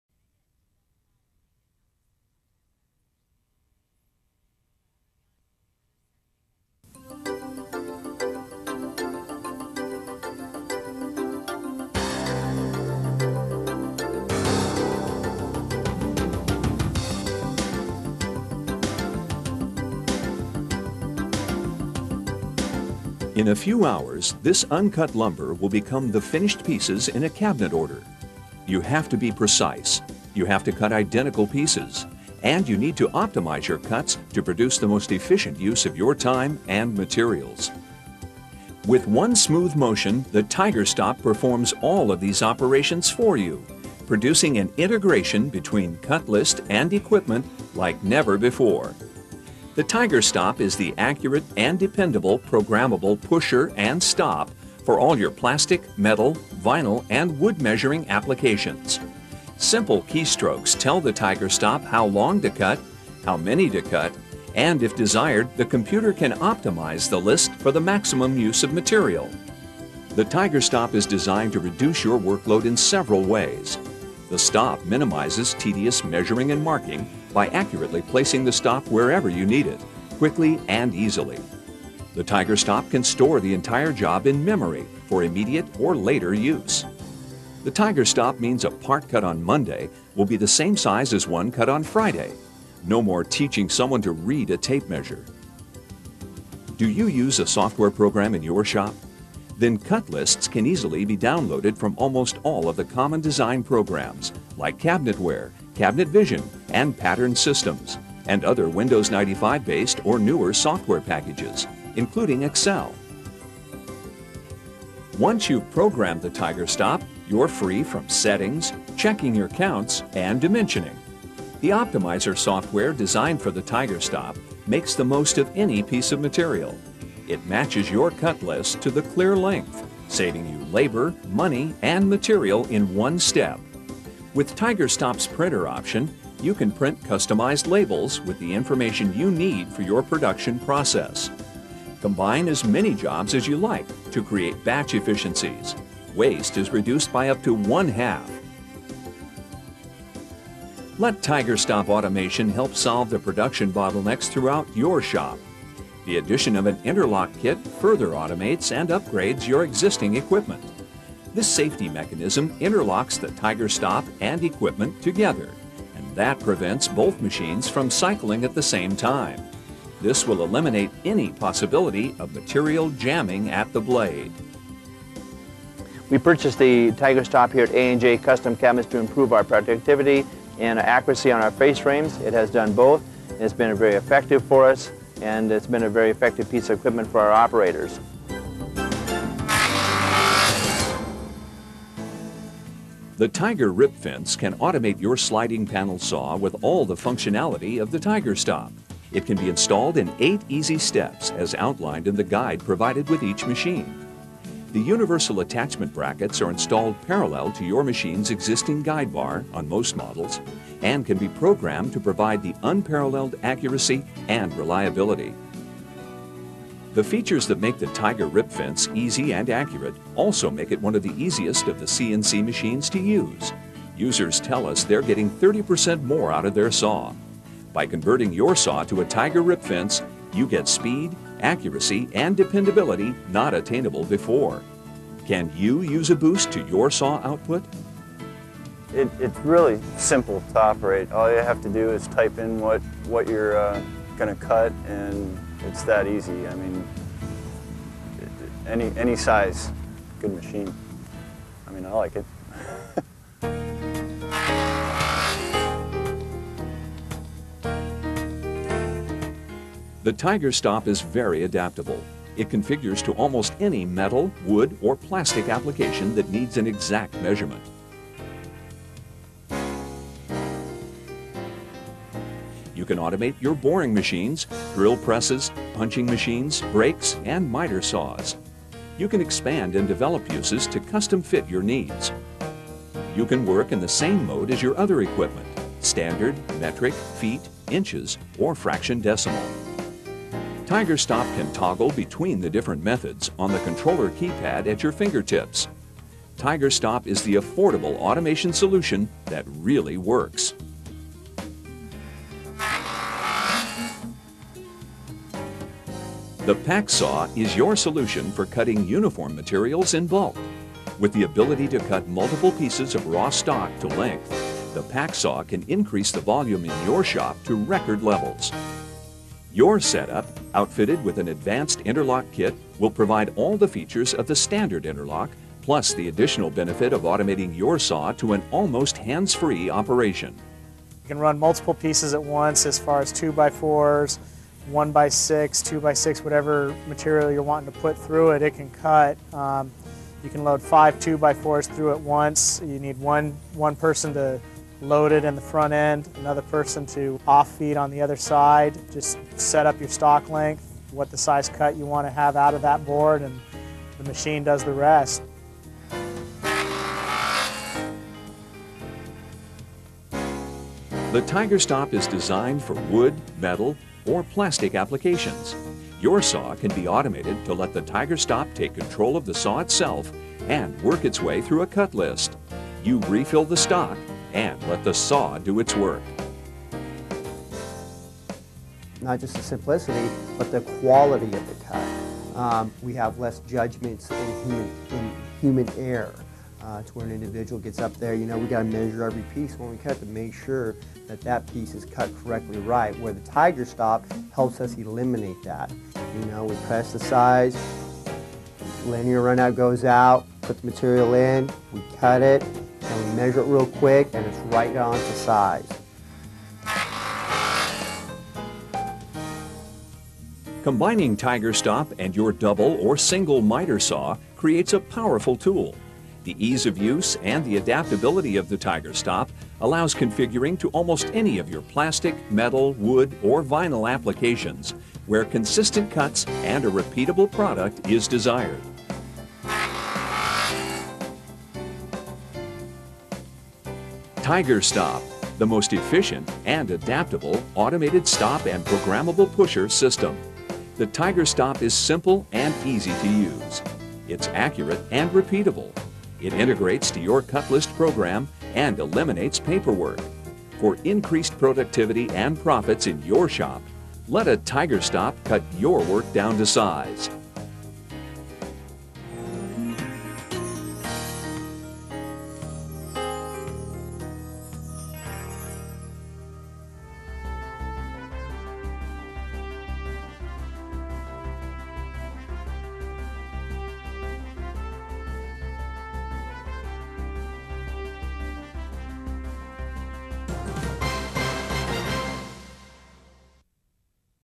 TigerStop Lineal Positioner for Metal and Woodworking
Lineal positioners are quickly becoming one of the most important pieces of equipment in the window fenestration industry.  The use of a top-of-the-line lineal positioner creates an automated stop/gauge and pusher system in which employees can be quickly trained, generally within an hour, creating a massive efficiency across manufacturing areas, by helping to move product through fast and easily.